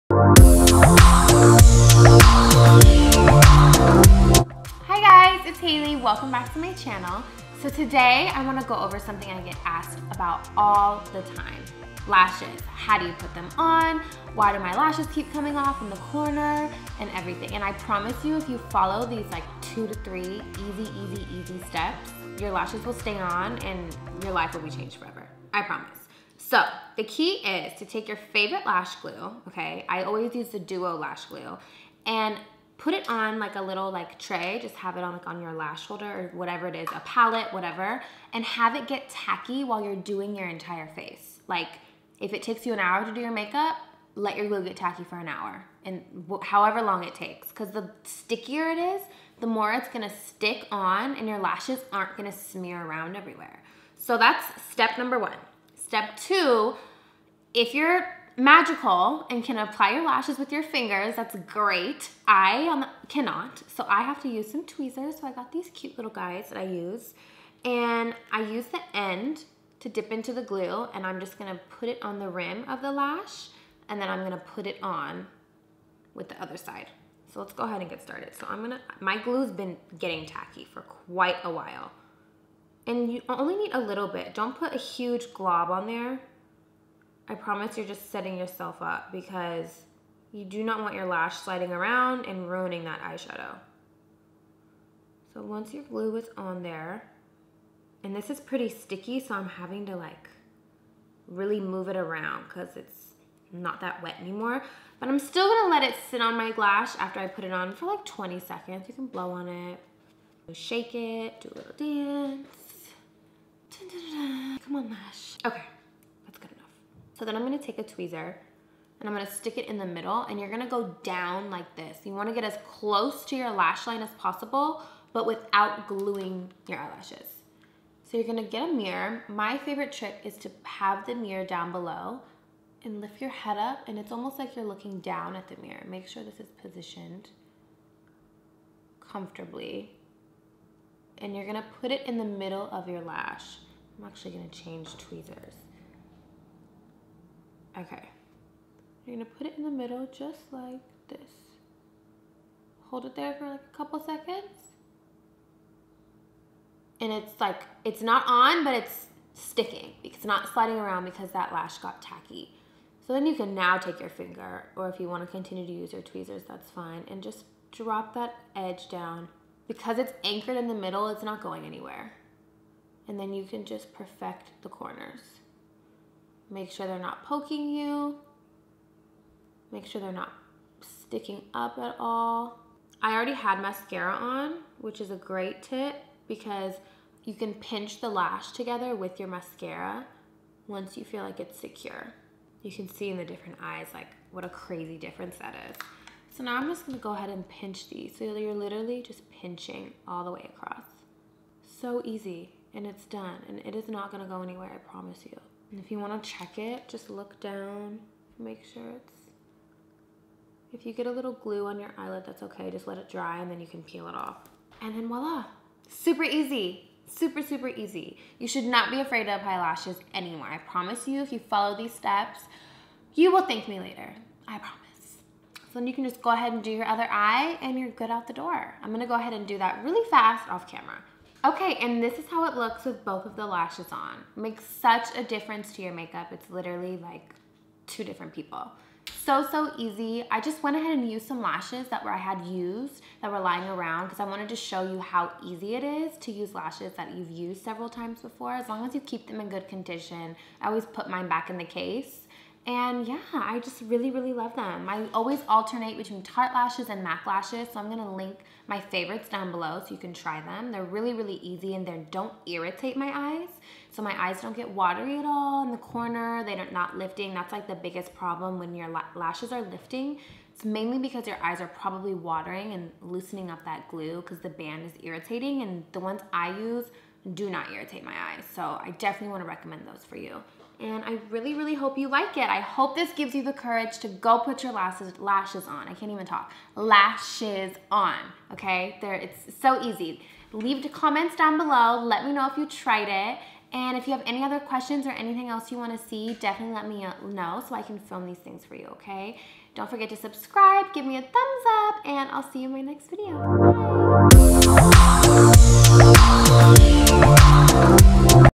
HOW TO APPLY FALSE LASHES FOR BEGINNERS | BEAUTY BASICS
Applying lashes is easy as 1-2-3 !! Watch and learn how now!

PRODUCTS MENTIONED

Tarte Lashes

MAC Lashes

SNAPCHAT: @haaaaays  (yes thats 5 A's)